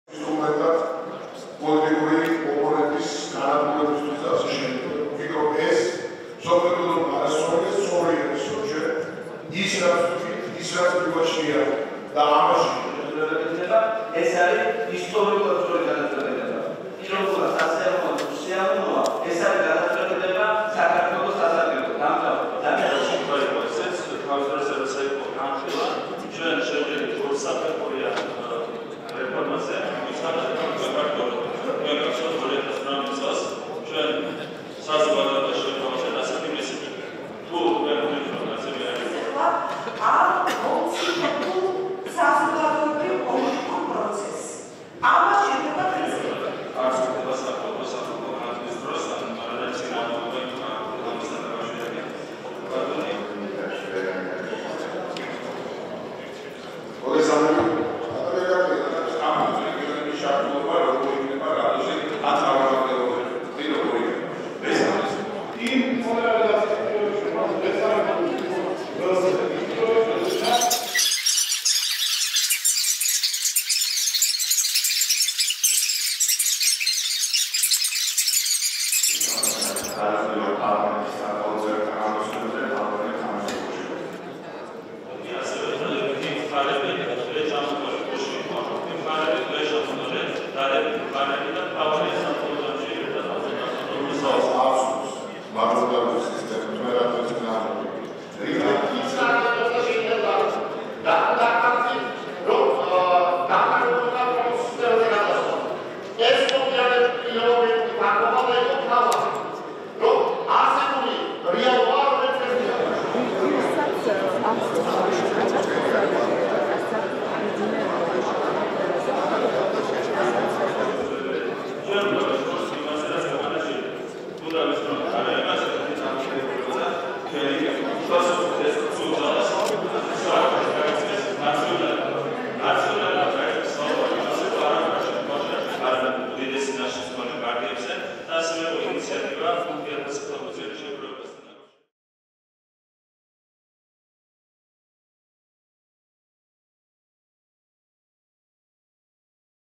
პარლამენტში კენჭისყრა გადაიდო
პარლამენტში გადაიდო საარჩევნო სისტემის შეცვლის შესახებ საკონსტიტუციო ცვლილებების კანონპროექტის კენჭისყრა.
რას აკეთებენ დეპუტატები, როცა სიტყვით არ გამოდიან?